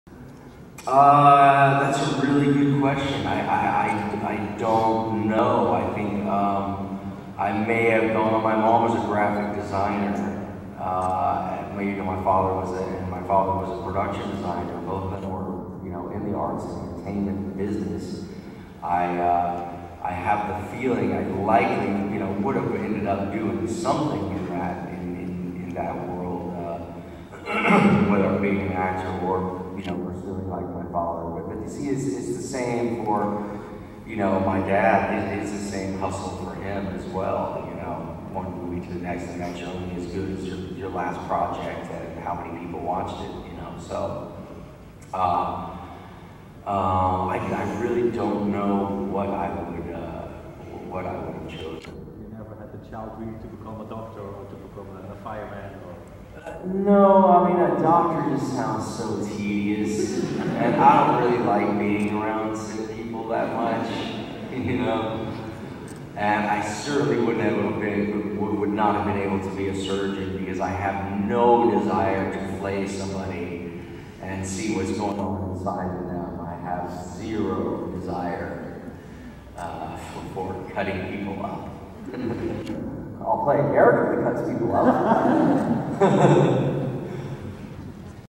Corin Nemec about being a surgeon
Corin Nemec at FACTS 2017 talking about how he would not be a good surgeon...